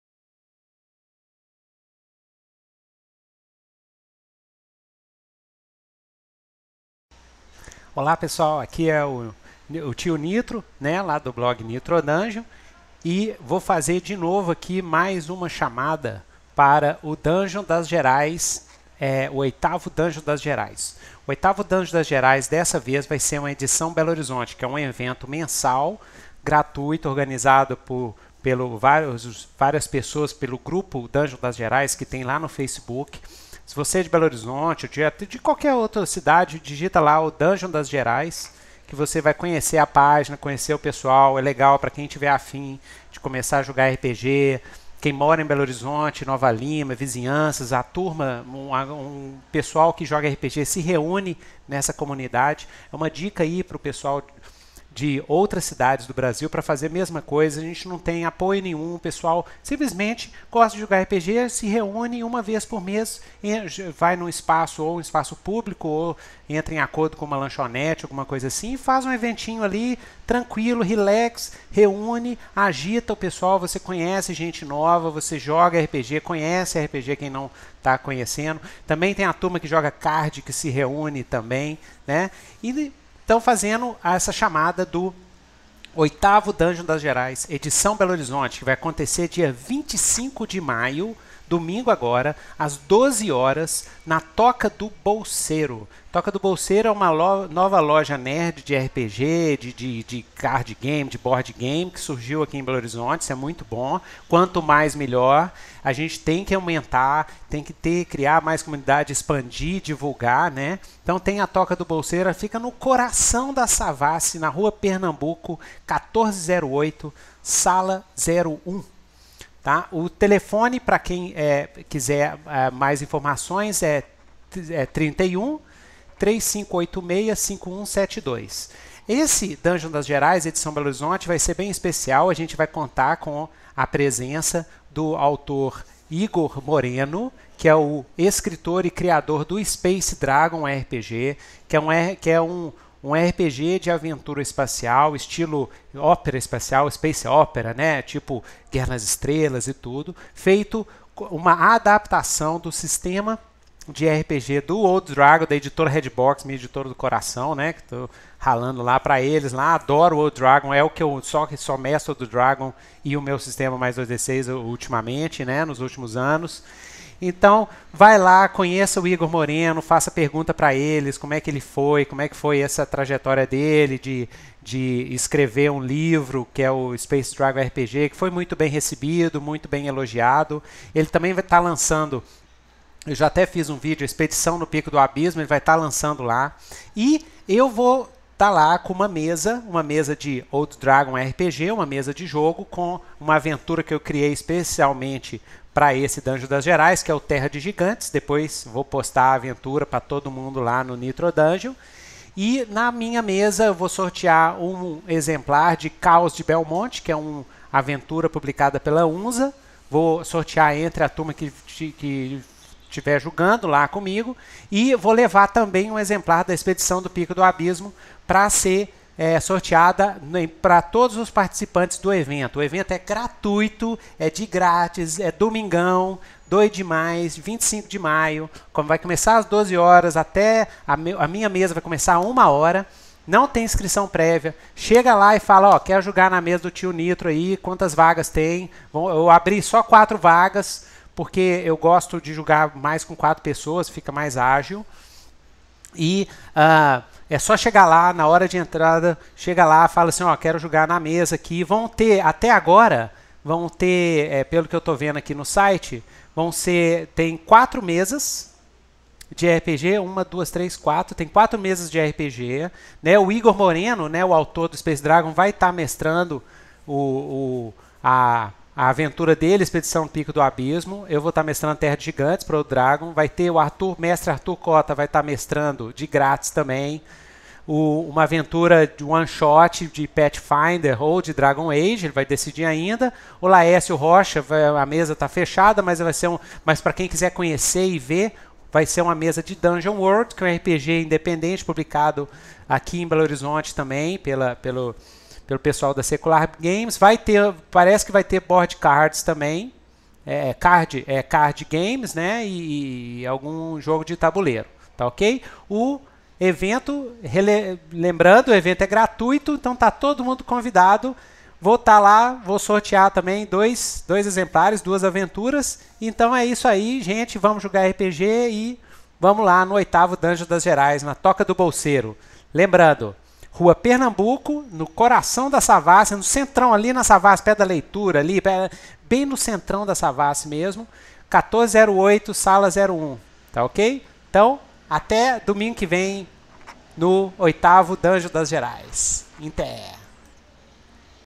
NitroVideo: Venha jogar RPG no 8ºDungeon das Gerais - BH (25/05/14)
JOGUE COM O TIO NITRO NO 8º DUNGEON DAS GERAIS (25 de Maio, 2014, Pernambuco 1408,Savassi BH)! - Vou estar no 8º Dungeon das Gerais (que contará com a presença de Igor Moreno, autor de Space Dragon e Expedição ao Pico do Abismo da Redbox Editora ).

O oitavo Dungeon das Gerais vai acontecer na Toca Do Bolseiro, dia 25 de maio (domingo) na Rua Pernambuco, 1408 - Sala 101, no coração da Savassi, o dia inteiro!

Minha mesa de Old Dragon (com sorteio de brindes) começará às 13:00 em ponto (e irá até 15:00) inscrições na hora por ordem de chegada!

Vou mestrar uma aventura para Old Dragon feita especialmente para o evento, a "Terra de Gigantes" e vou sortear um exemplar de "Caos em Belmont" da Editora UNZA RPG (aventura de horror para Old Dragon) e um exemplar da "Expedição ao Pico do Abismo" para os participantes do Evento!

Compareça véi!

Dia 25 de maio na Loja de RPG Toca do Bolseiro, Rua Pernambuco, 1408 - Sala 101. Savassi.

Mesas de RPG no Evento:
RPG:
Mestre - Aventura - Sistema/Cenário
Igor Moreno - Expedição ao Pico do Abismo - Space Dragon/Old Dragon
Newton Nitro - Terra de Gigantes - Old Dragon (início 13hs)
Arthur Cota - Aventura One Shot - Pathfinder ou Dragon Age
Laercio Rocha - Nostalgic Portals - DungeonWorld (Mesa Fechada)